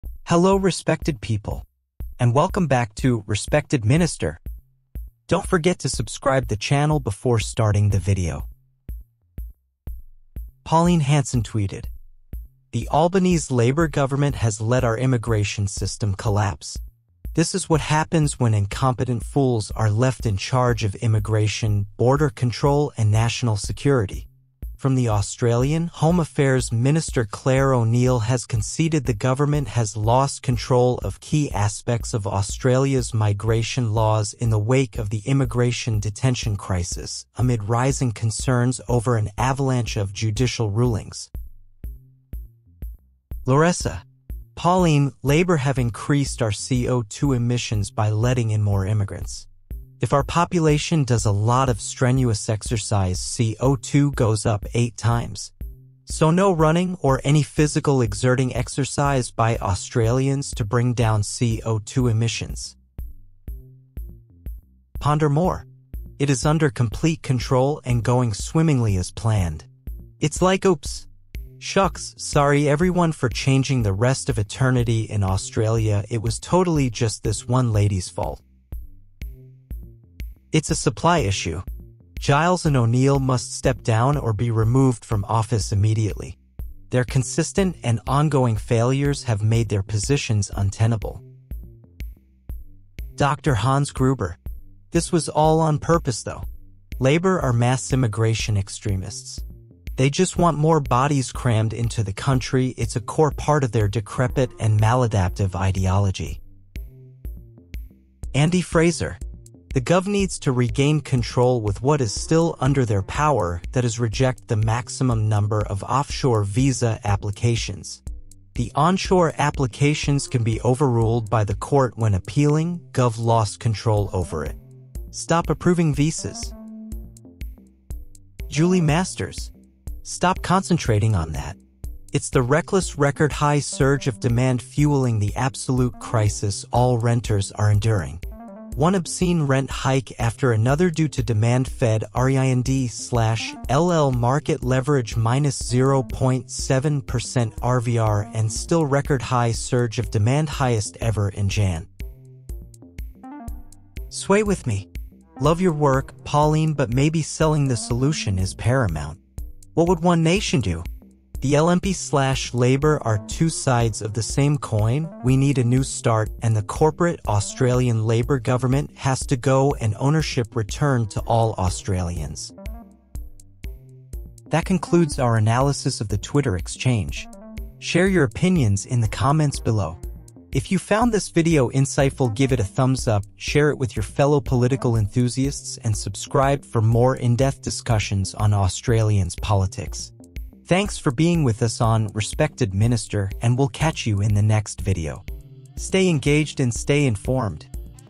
Pauline Hanson Sounds the Alarm on Immigration Detention Crisis! Australian Labor ! Liberal , Greens
Respected Minister community, welcome to a thought-provoking episode where we delve into Pauline Hanson's critique of the Albanese Labor government's handling of Australia's immigration system. In her tweet, Hanson raises concerns about incompetence and lack of control in immigration, border control, and national security. Let's unpack this complex issue together!

🔍 Join us as we analyze the implications of Home Affairs Minister Clare O'Neil's concession regarding the government's control over migration laws, amidst an immigration detention crisis and judicial rulings. With insights from The Australian, we explore the challenges facing Australia's immigration system and the need for effective governance.

🤔 What's your perspective on Pauline Hanson's critique of Australia's immigration management? Do you agree with her concerns, or do you have a different view? Share your insights below!

Let's keep the dialogue alive, because informed citizens drive positive change in a resilient democracy! 🌟 ALP ! LNP ! Australian Labor Party ! Liberal Party of Australia ! Australian Greens Party